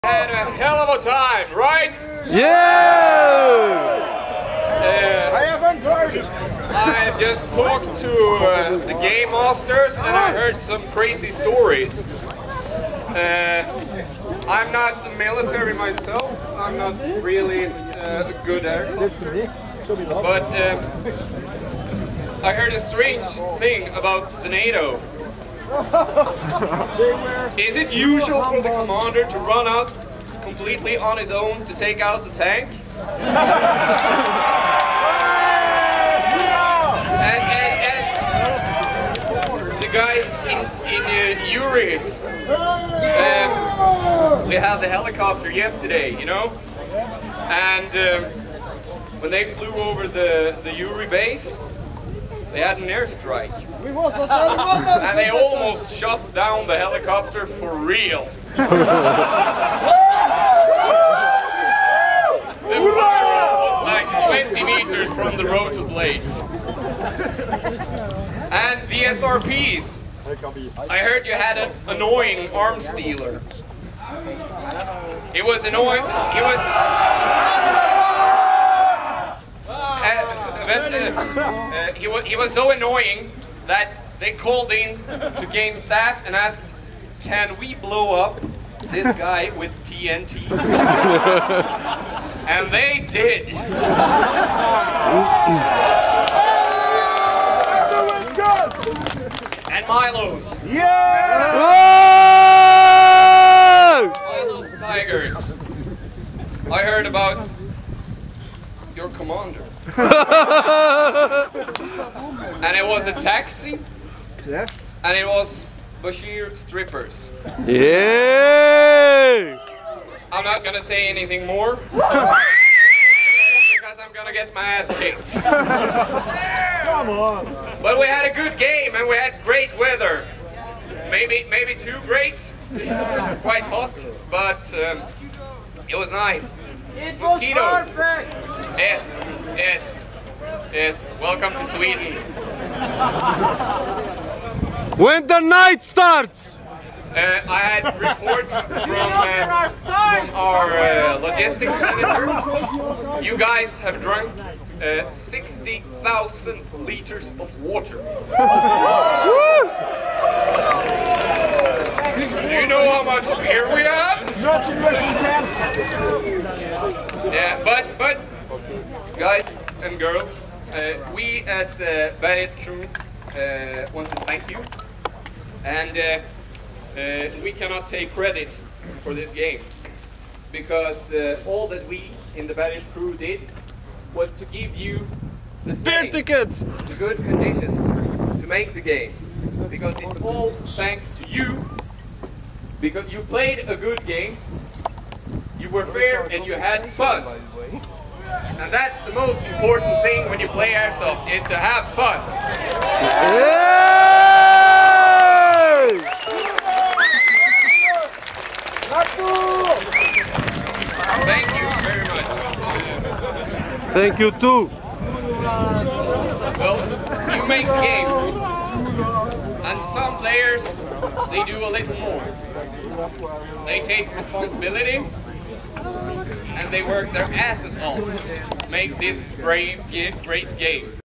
BERGET 7 - ending speech
Ending speech during the beer party